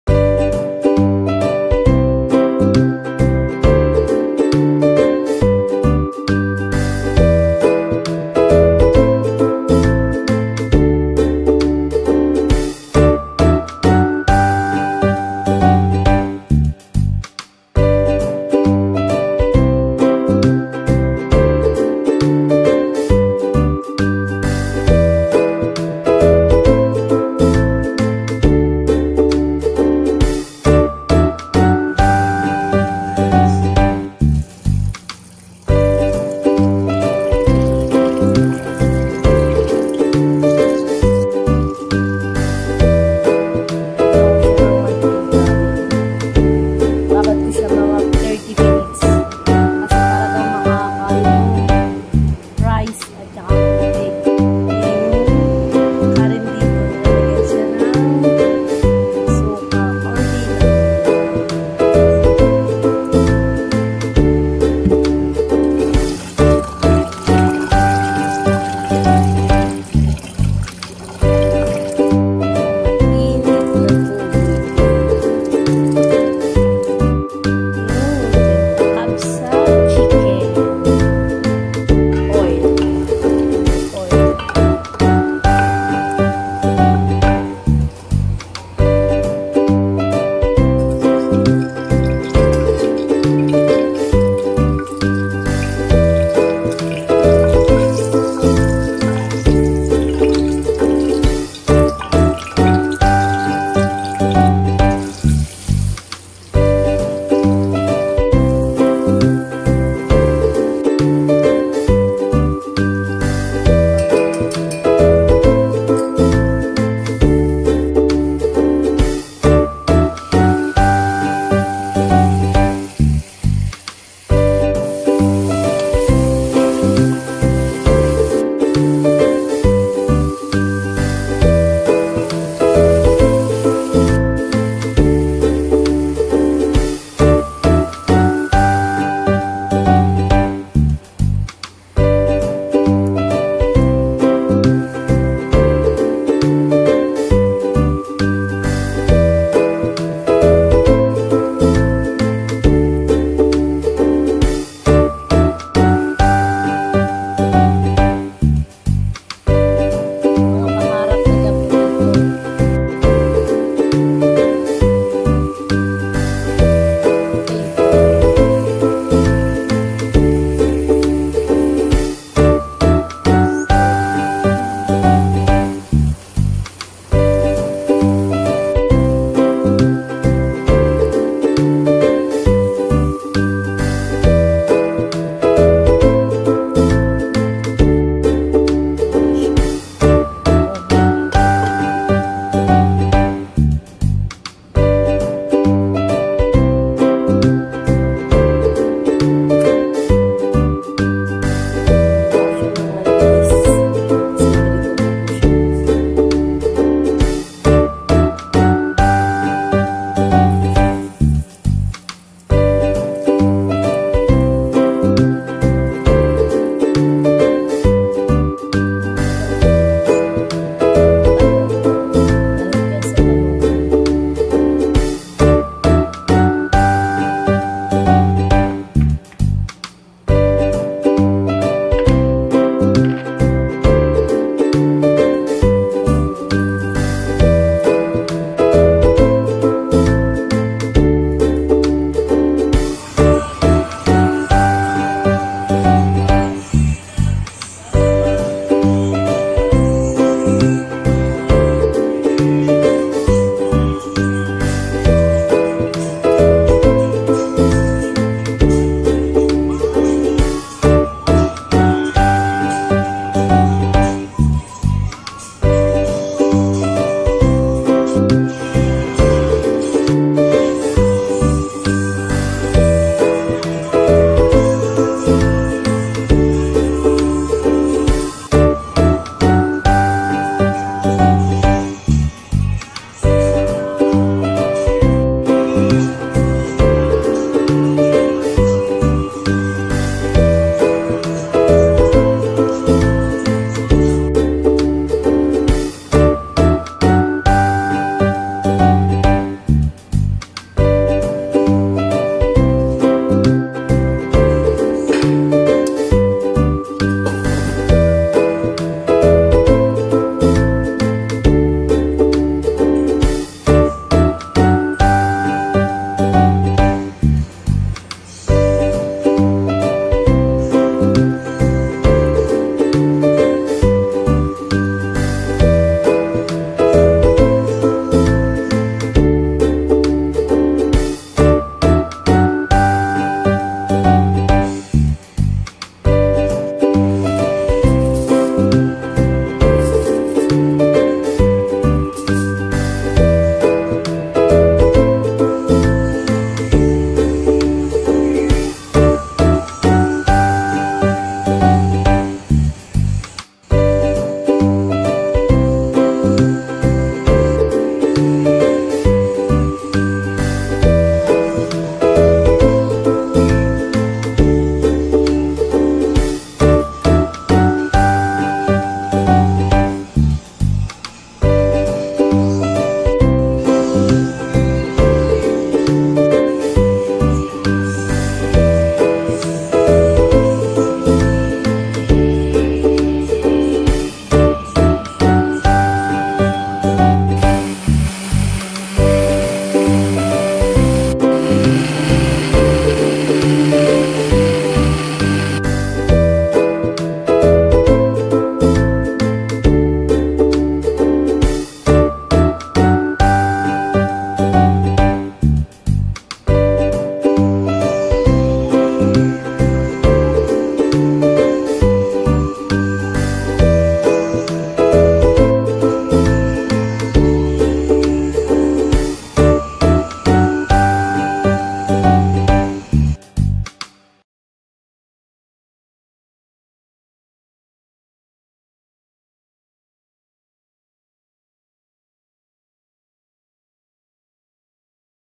How I Prepare lunch for my Employer
Chicken Kabsa
Eggplant w/chizz
Salad
Lemon juice w/mint